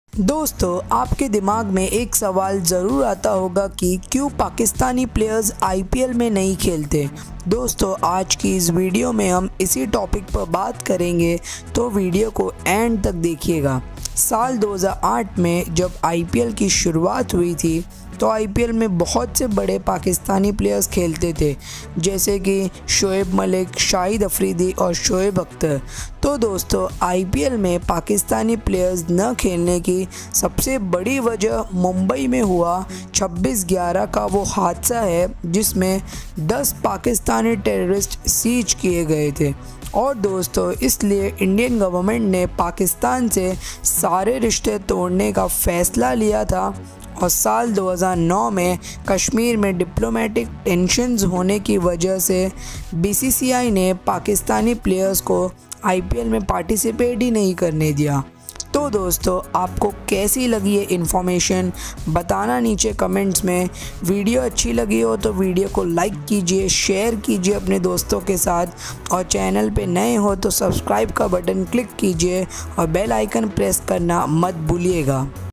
Why Pakistani Cricketers are Not Allowed to Play in IPL? | RS Cricket Spot
Doston Aaj Ki Iss Video Me Maine Aapko IPL mai Pakistani Cricketers Kyun Nahi Khelte Iska Reason Bataya Hai Ummeed Hai Ki Aapko Ye Video Bahut Pasand Aayegi | Thankyou so much...